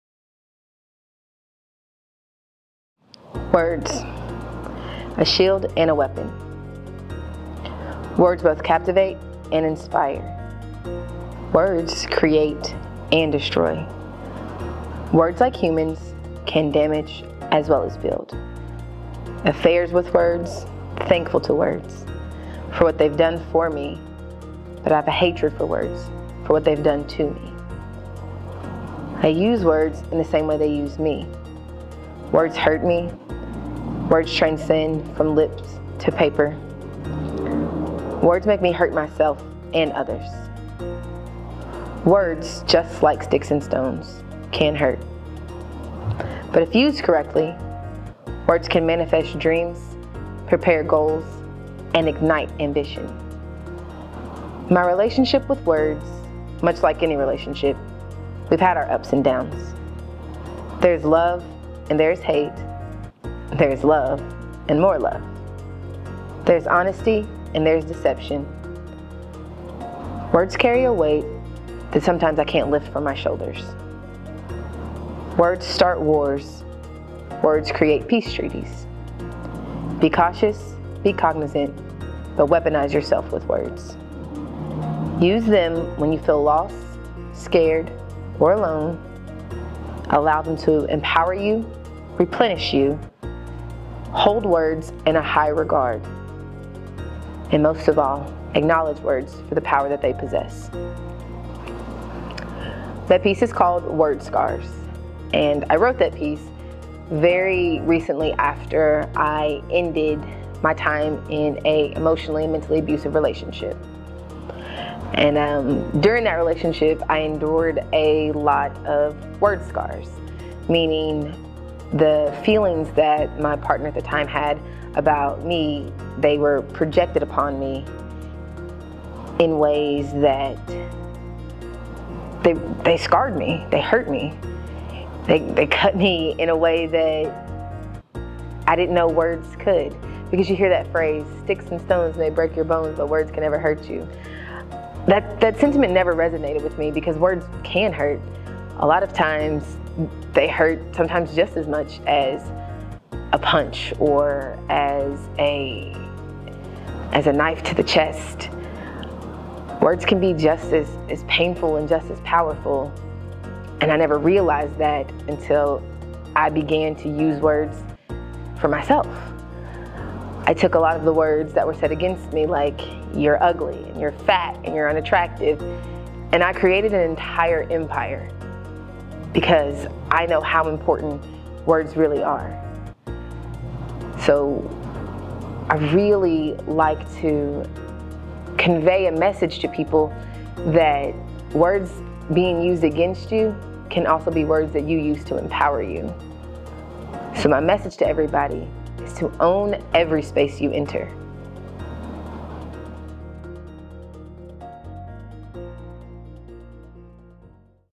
Word Scars
Fairen Kia reading her poem Words from her self-published second book, untitled and unreleased. Check out the Love Thy Belly Podcast.
Production: TNG Video